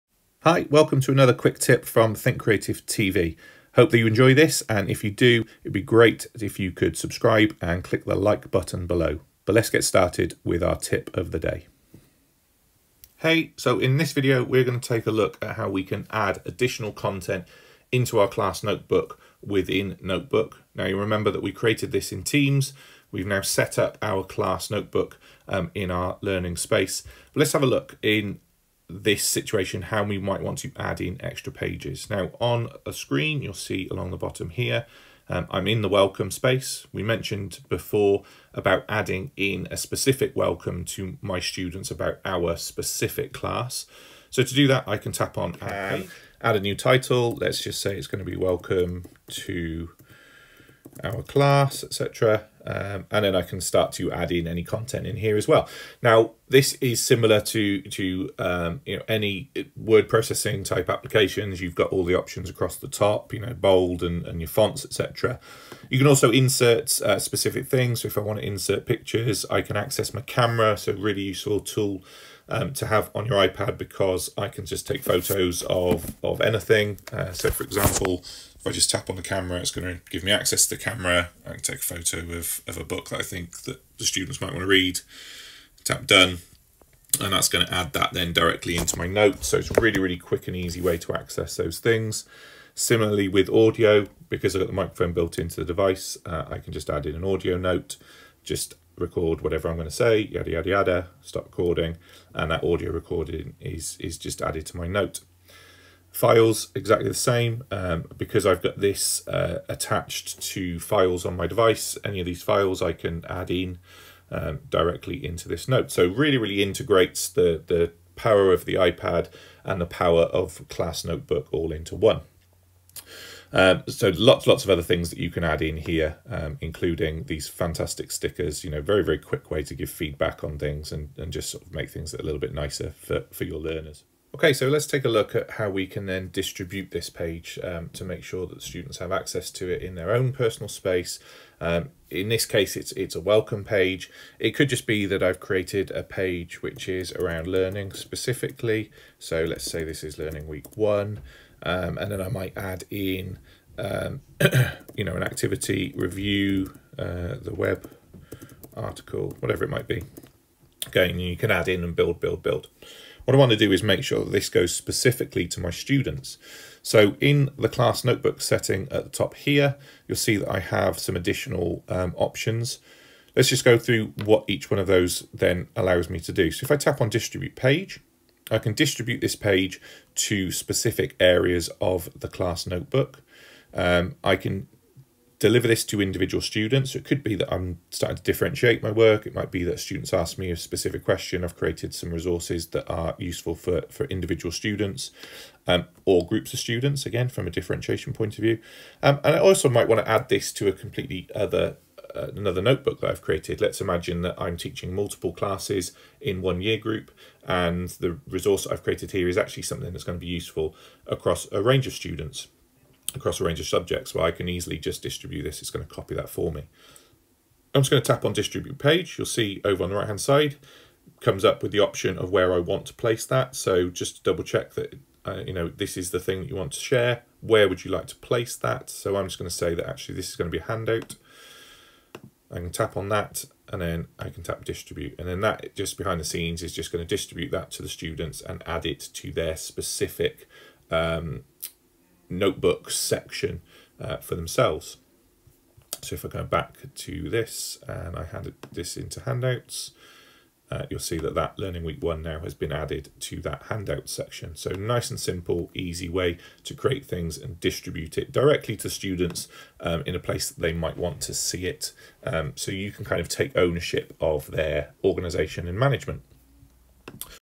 Teams Tips: Adding Pages in Class Notebook
In this video we take a look at how you can add additional content to your Notebook and distribute it to the students.  This helps in developing content and making sure its in a place where your students can see it.